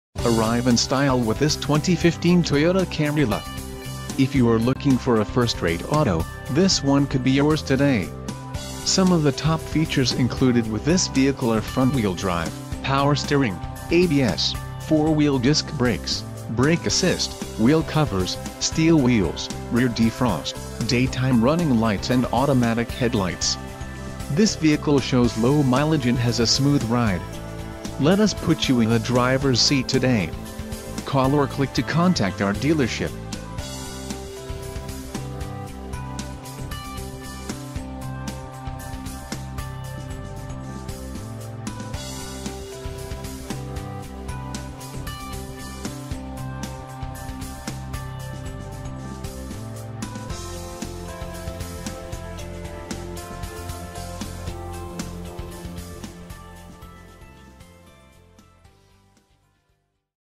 2015 Toyota Camry LE - Stadium Toyota - Tampa, FL 33614
Introducing our bold new 2015 Toyota Camry LE in Celestial Silver Metallic. A 2.5 Liter 4 Cylinder engine pairs with a smooth-shifting 6 Speed automatic transmission and generates 178hp on demand. This efficiency rewards you with nearly 35mpg on the open road. Solid engineering and high-grade workmanship continue to earn this car accolades every year!    The exterior of the LE is accented by a dynamic shape, automatic headlamps and distinctive tail lights. The ergonomically designed interior features a 6.1-in. touch-screen Entune Audio display with Bluetooth phone and a six-speaker sound system with steering wheel mounted controls. You'll also appreciate remote key-less entry, cruise control, a rearview camera, a tilt-and-telescoping steering wheel and split-folding rear seats. Confident handling, straightforward technology and lots of comfortable passenger space make this car an absolute delight to own.